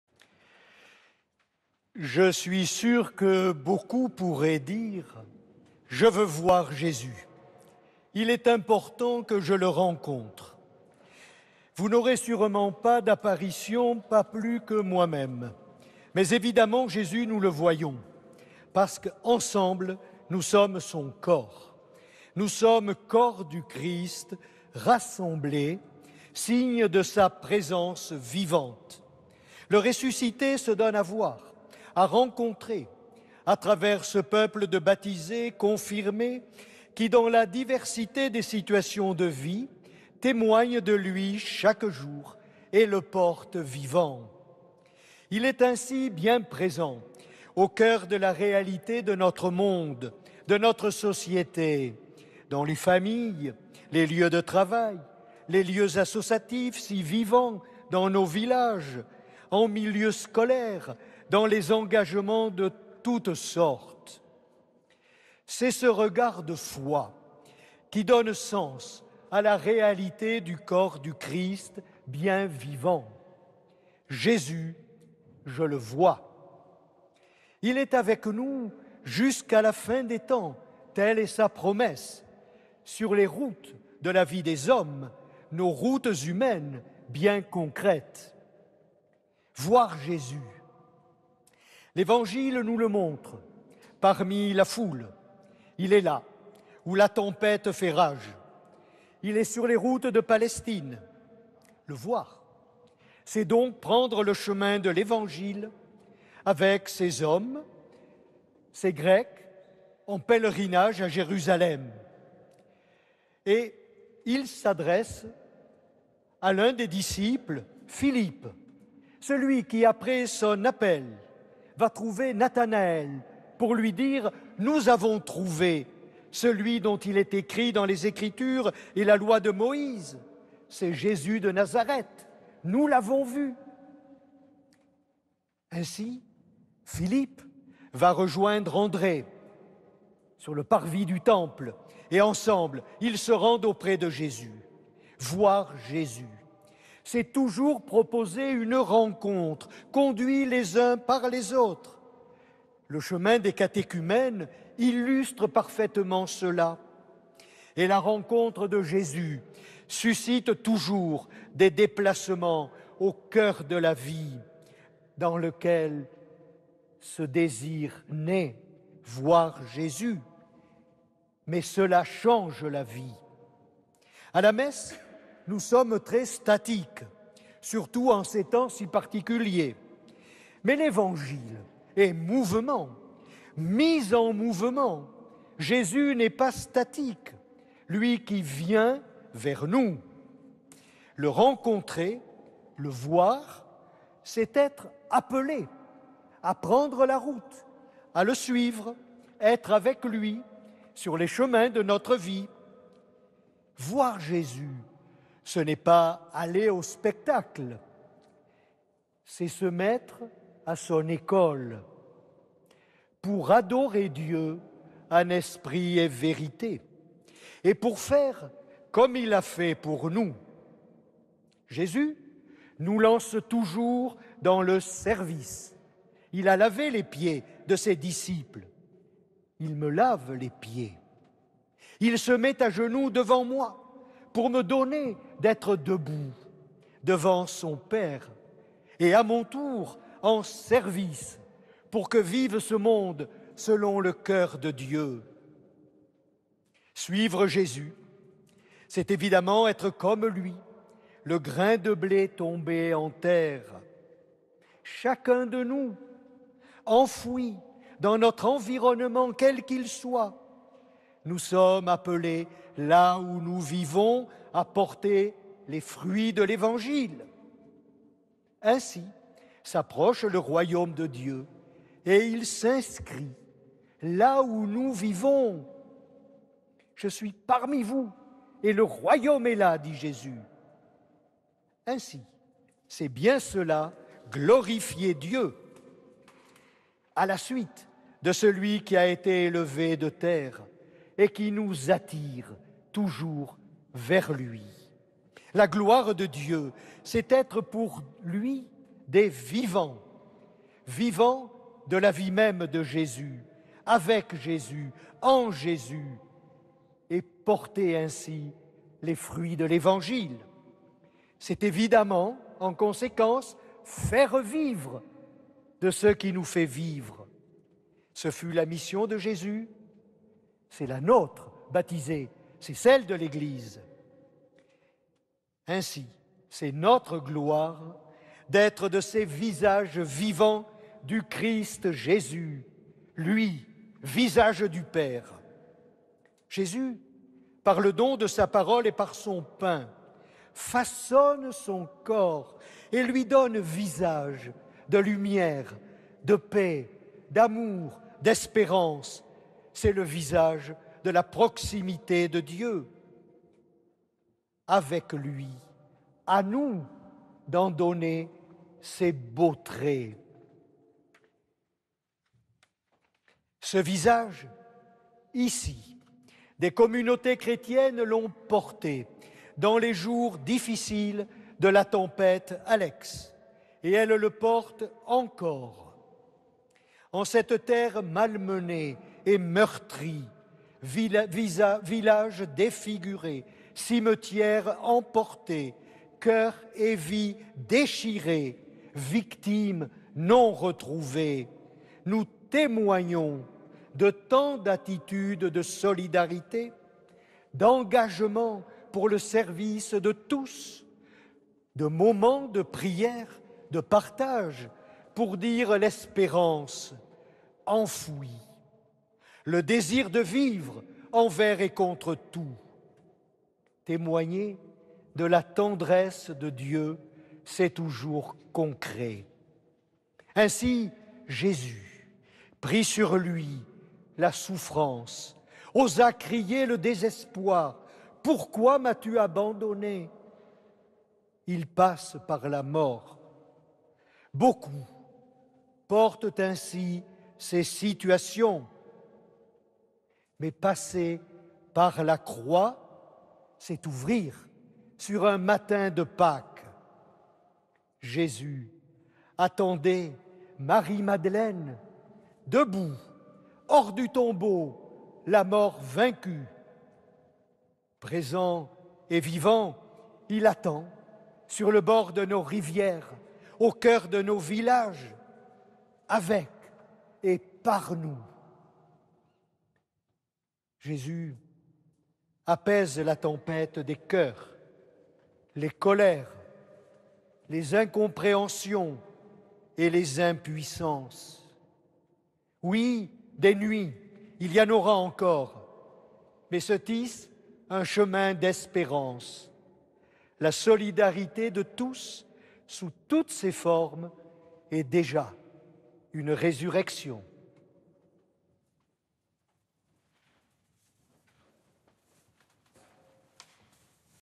Voir Jésus, un chemin d'espérance | Homélie du dernier dimanche de Carême à Sospel
« La solidarité de tous, sous toutes ces formes, est déjà une résurrection. »

Revivez la lecture de l'homélie de la messe du 21 Mars 2021, prononcée par Mgr André Marceau, évêque de Nice.